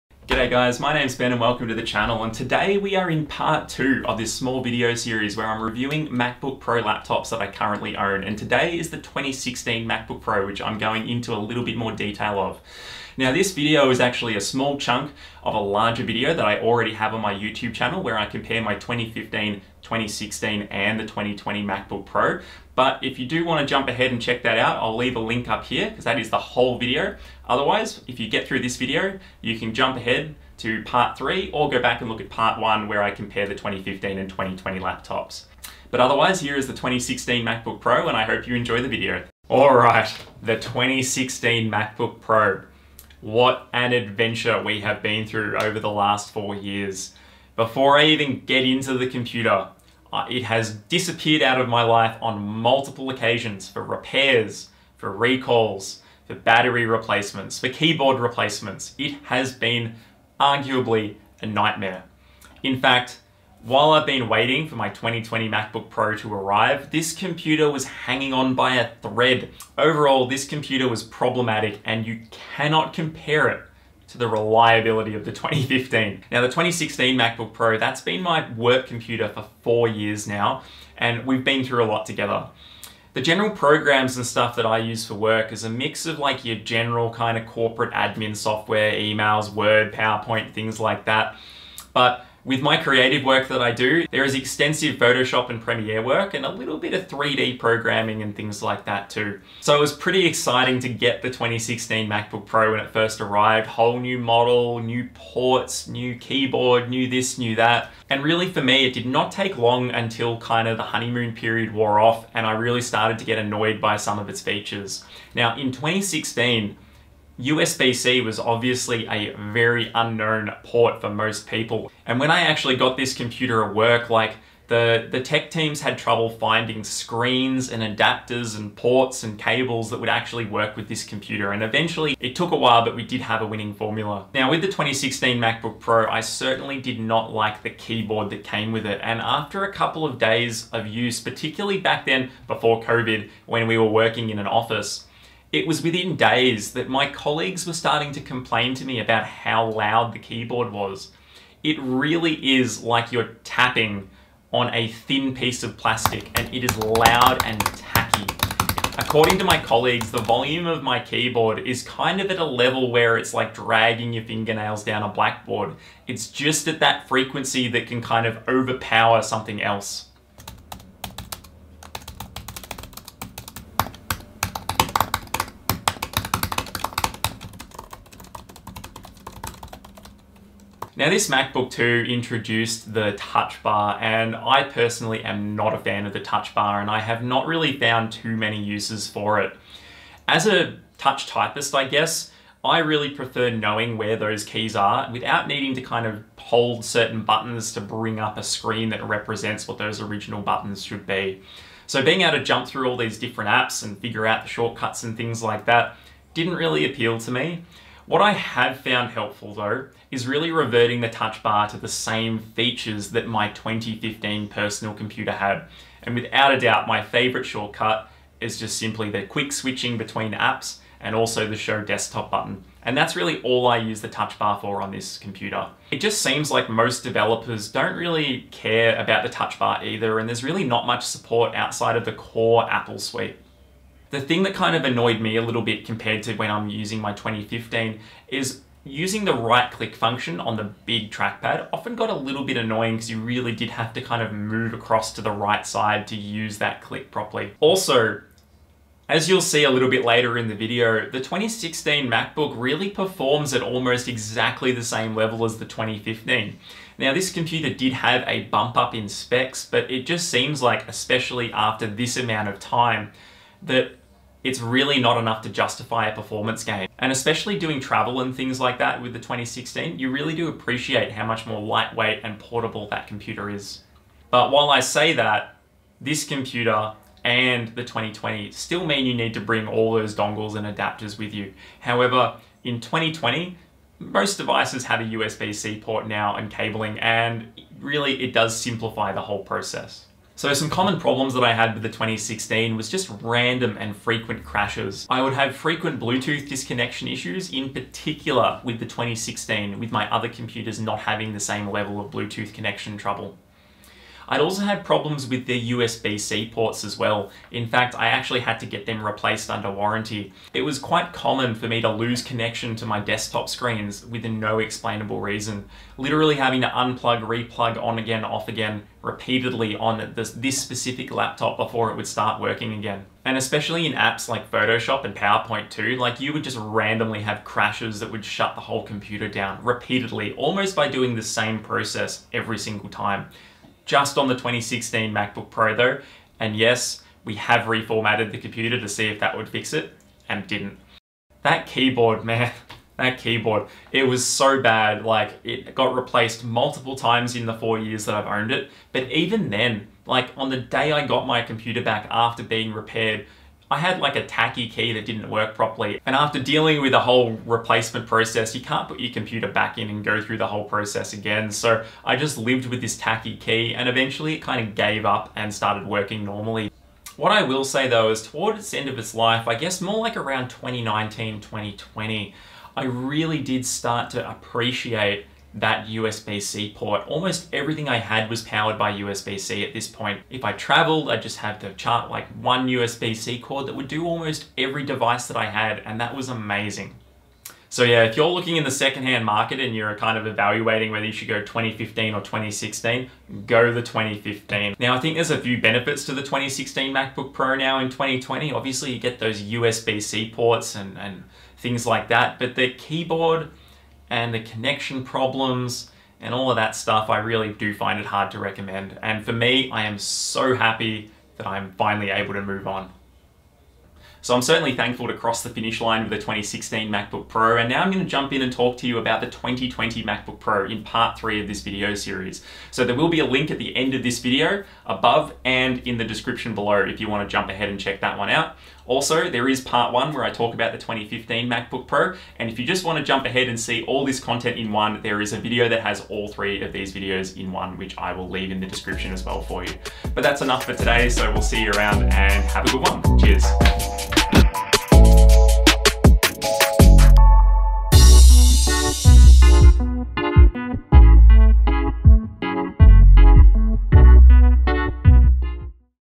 2016 Macbook Pro 15" Owner Review | Is the first of its generation worth it in 2020?
I've owned the Apple 2016 Macbook Pro 15" since new & in this long term review I'll chat about my experiences with the original Apple 'donglebook pro' laptop and how it compares to newer and older Macbook Pro computers that I still use just as much. More info in the description on this 3 part series:

LINK TO PRODUCT:

Timeline Chapters: 2016 15 inch Macbook Pro
00:00 - Introduction
00:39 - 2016 - Macbook Pro 15” Intro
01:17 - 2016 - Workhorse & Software
01:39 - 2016 - Day 1, New Generation
01:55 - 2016 - USB-C in 2016
02:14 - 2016 - Butterfly Keyboard Experience
02:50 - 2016 - COMPARISON Typing Volume
03:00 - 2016 - Touch Bar Experience
03:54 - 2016 - Large Touch Pad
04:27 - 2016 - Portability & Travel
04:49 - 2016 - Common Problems
05:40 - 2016 - Butterfly Keyboard Problems
06:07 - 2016 - When USB C Got Good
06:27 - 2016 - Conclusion

Written Summary:
Hey all, welcome to my long term ownership review of the first and original Apple Donglebook Pro, I mean, the 2016 Macbook Pro 15". It has certainly been an interesting experience being one of the first to adopt USB-C ports, and experience the sometimes infuriating butterfly keyboard with all the recalls and repairs.  I actually have the 2015, 2016, and 2020 Macbook Pro's at the moment so I thought I would put a small ownership and comparison video series together in the off chance it may help someone who is struggling to choose which models to buy. This is not a video where I jump into tech specs and benchmarks, I really just want to talk about the experience of using the computer. Everything I say here is said with love as an Apple fan :)

Camera:
Sony A7R3 w 24-105mm f4 lens